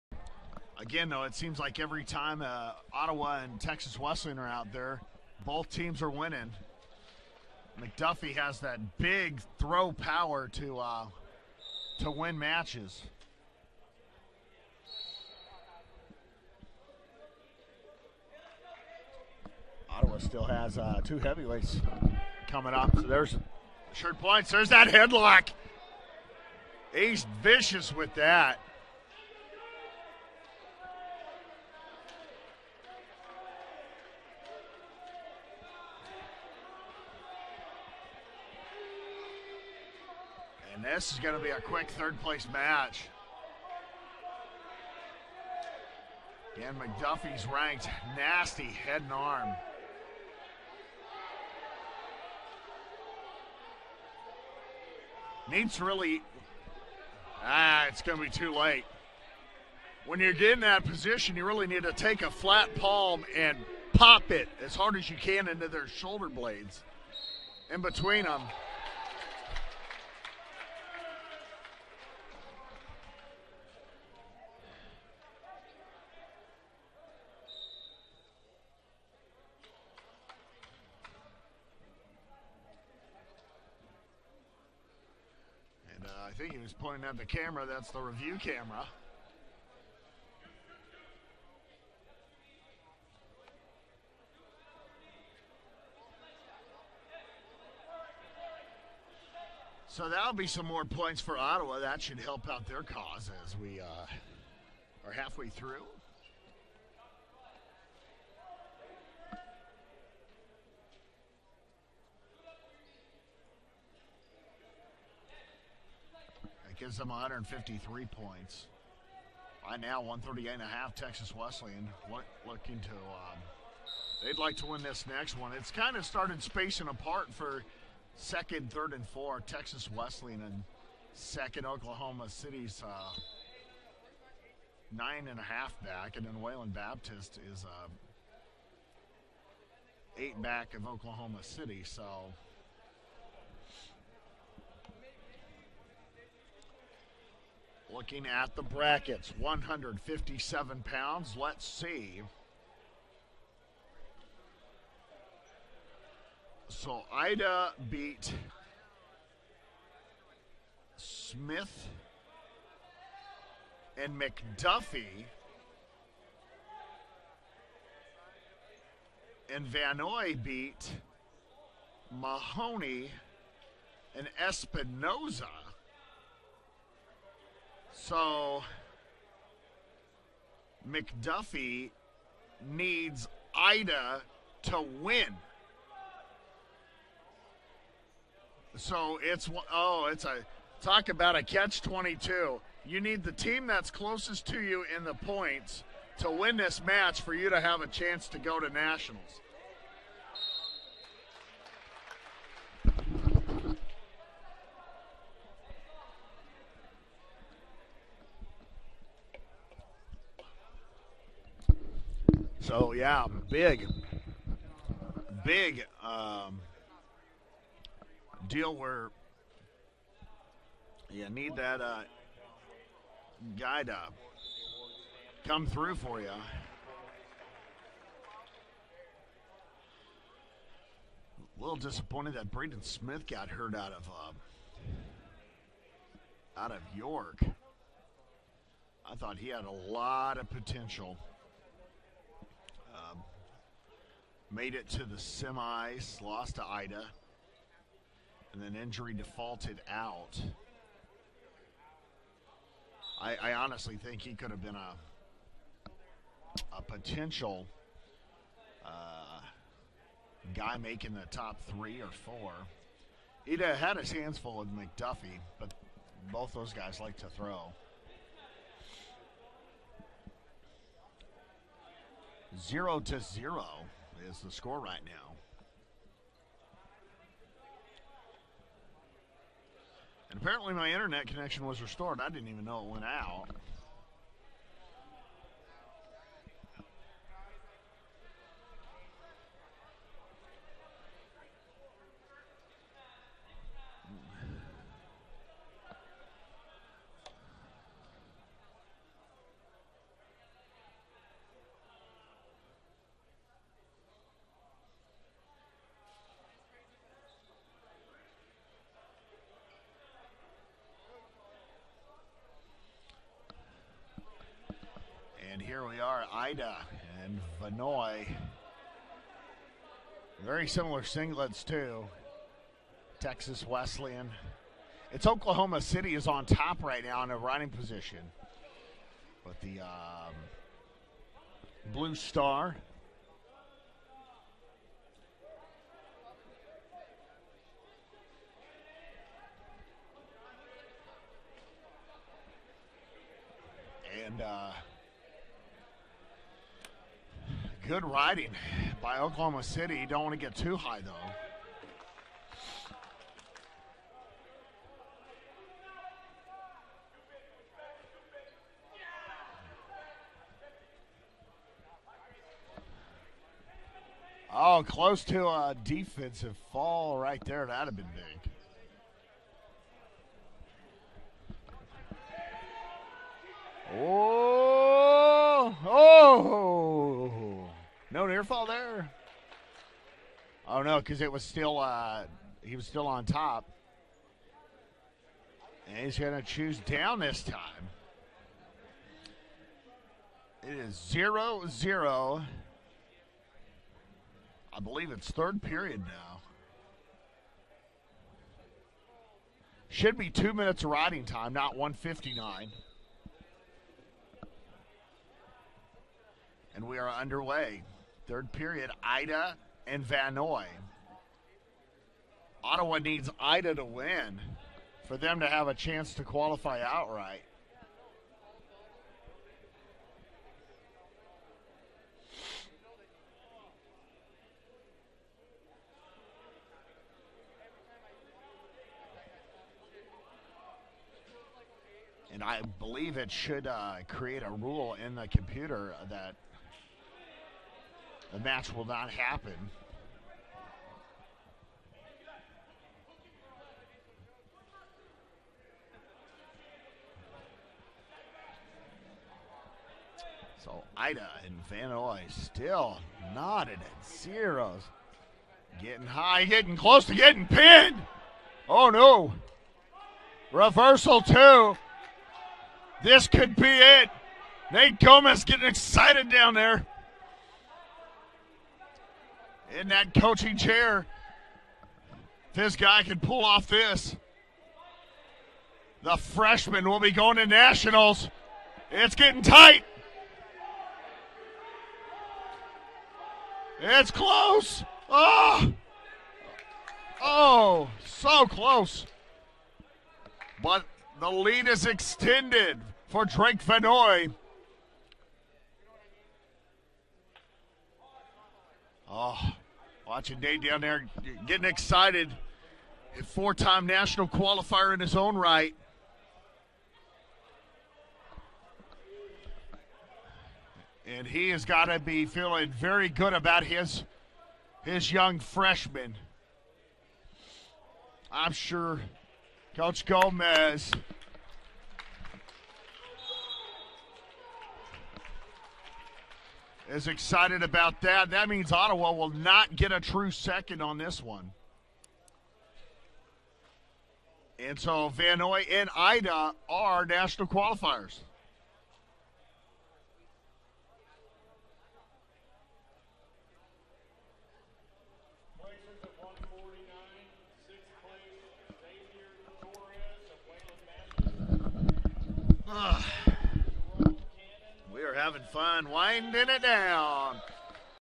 2021 157-pound KCAC-SAC men's wrestling championship final
Drake Vannoy from Oklahoma City University shut out Texas Wesleyan's Enis Ida 7-0 for the 157-pound title in the Kansas Collegiate Athletic Conference-Sooner Athletic Conference men's wrestling championships Feb. 13, 2021 at the Ed Pyle Sports Complex in McPherson, Kan.